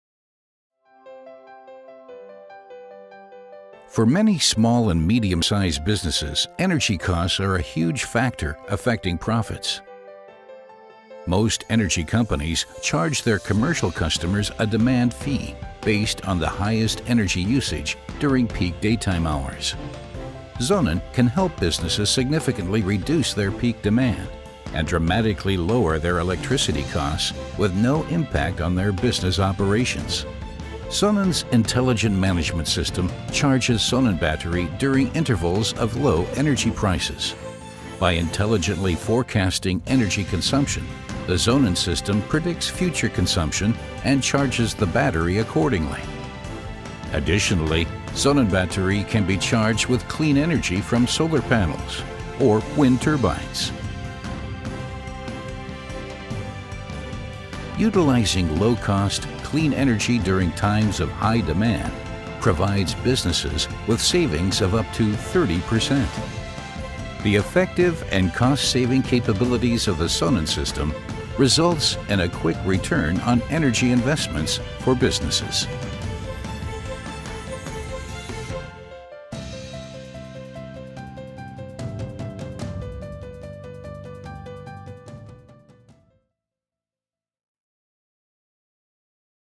Peak shaving with sonnenBatterie
Through intelligent energy management the sonnenBatterie helps businesses reduce their peak demand and energy costs without impacting business operations.